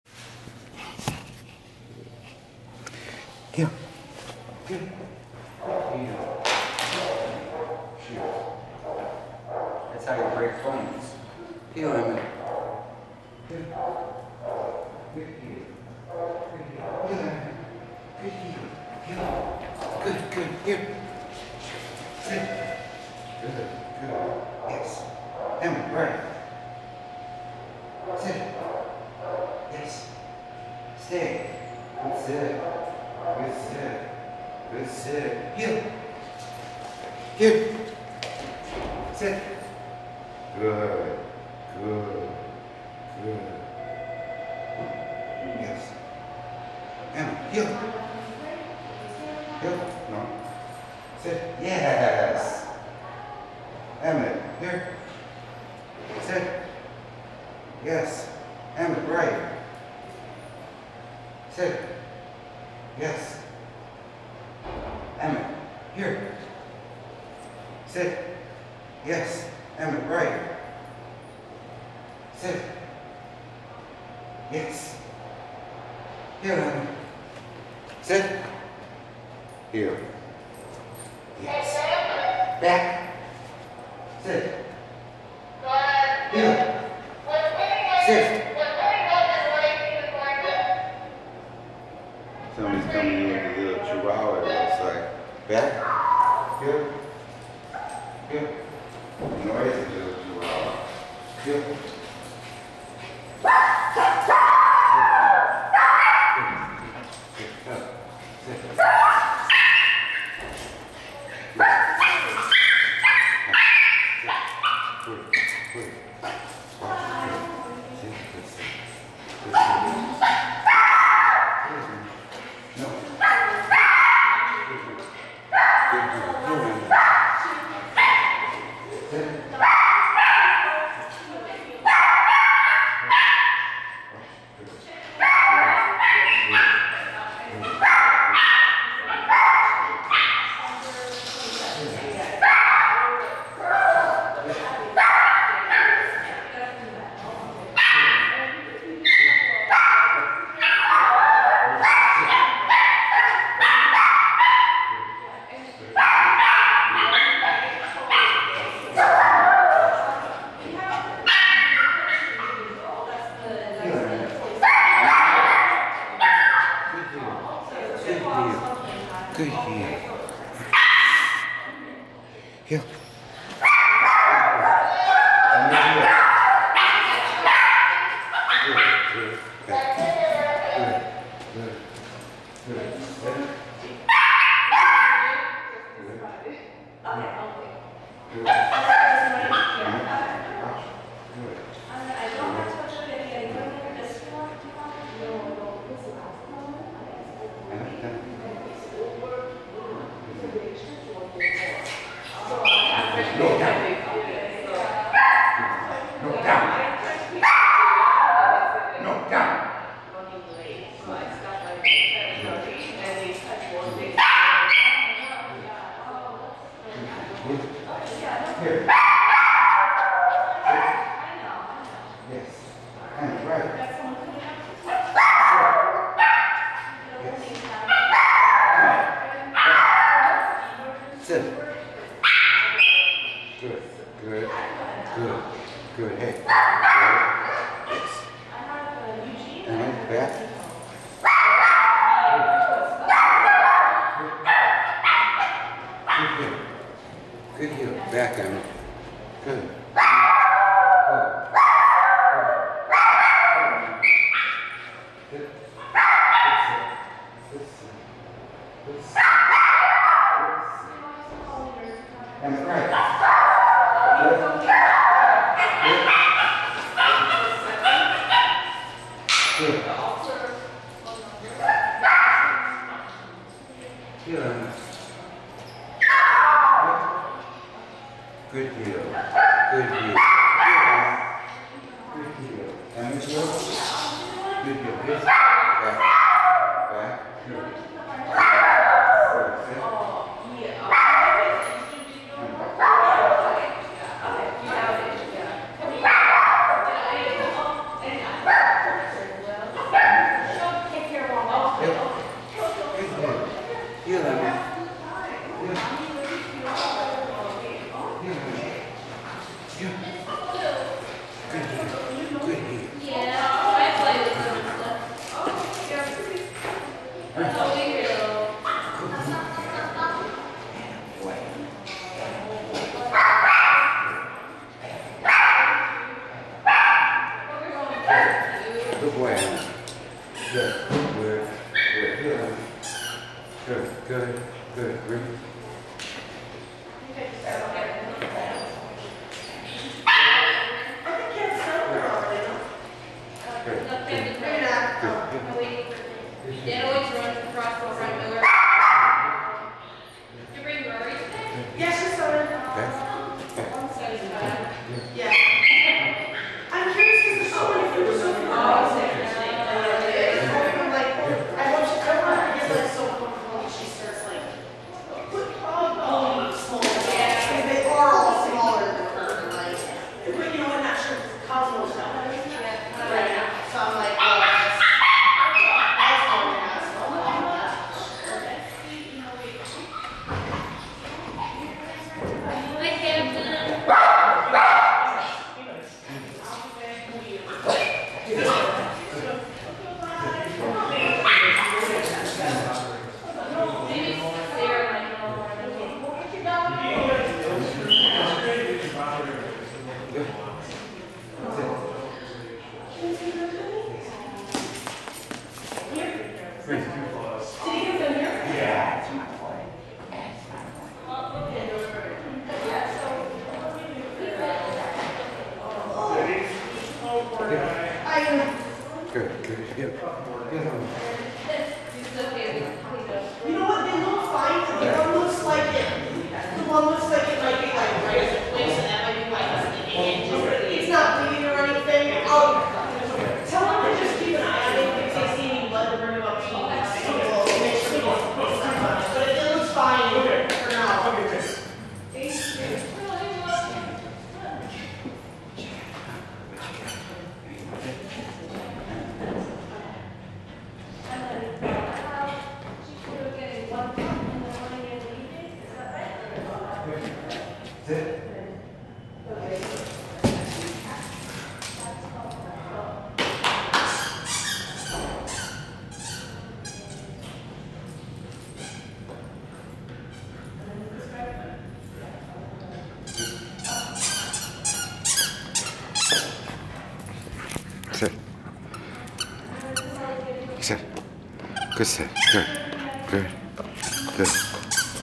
Allan Ross Dog Training
Emmett R. is a 5 month old Australian Labradoodle in a private session with me in the lobby.  Here we practice obedience near a very noisy little dog in the lobby.